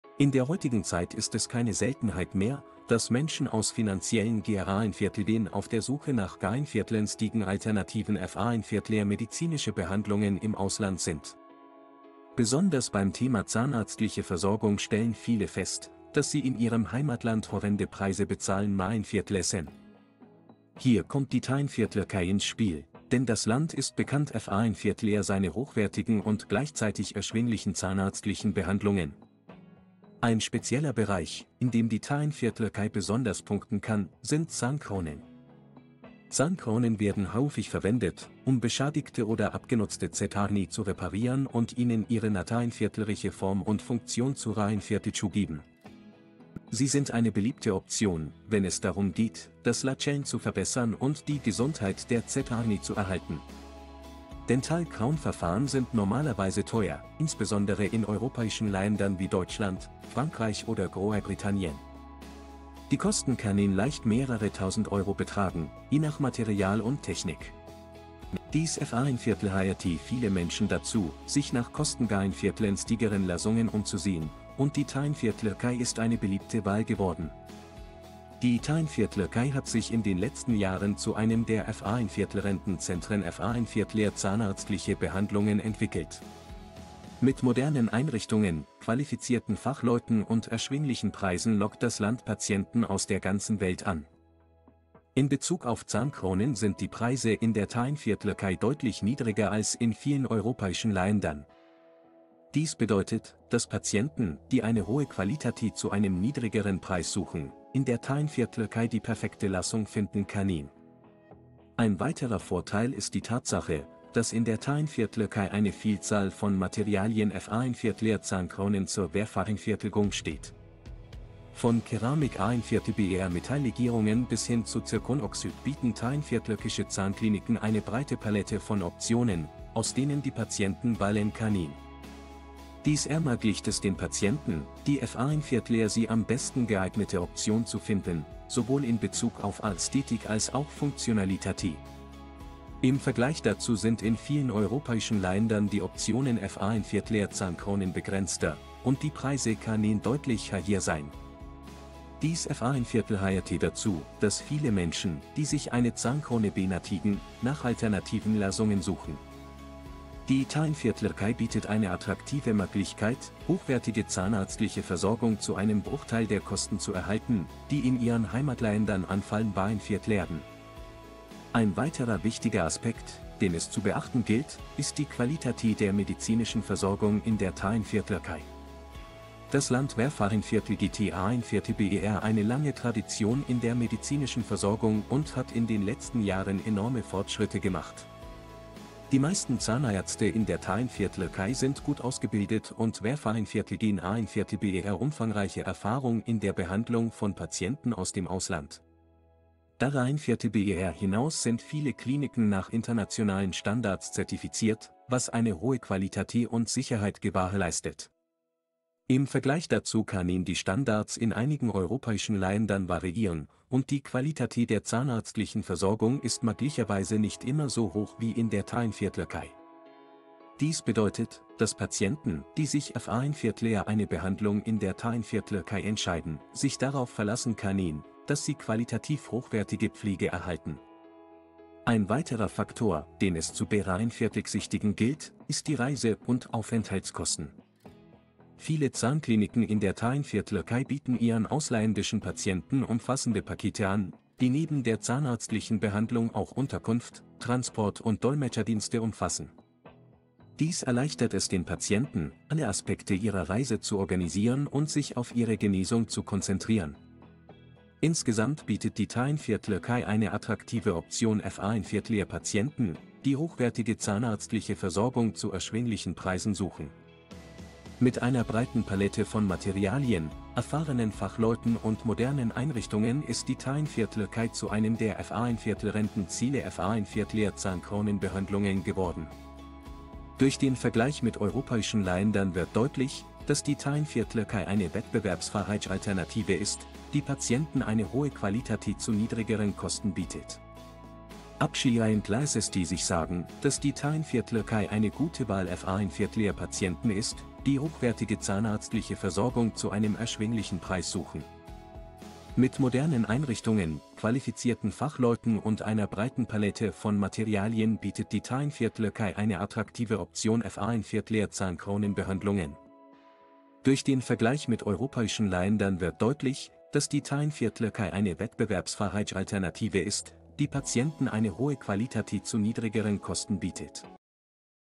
Günstige Zahnbehandlung im Ausland - Lohnt sich der Trip für meine Zähne? [100 Zeichen]
Kontaktiere uns:
Telegram In der heutigen Zeit sind viele Menschen auf der Suche nach günstigen Alternativen für medizinische Behandlungen im Ausland. Besonders bei zahnärztlichen Versorgungen ist dies oft der Fall. In vielen Ländern, wie zum Beispiel Ungarn oder Polen, sind die Kosten für Zahnersatz und andere zahnärztliche Eingriffe deutlich günstiger als in Deutschland. Viele Patienten entscheiden sich daher dazu, ihre Zahnbehandlungen im Ausland durchführen zu lassen, um Kosten zu sparen. Jedoch sollten vor einer solchen Entscheidung immer sorgfältig die Qualität der Behandlung und die Erfahrung des Zahnarztes überprüft werden.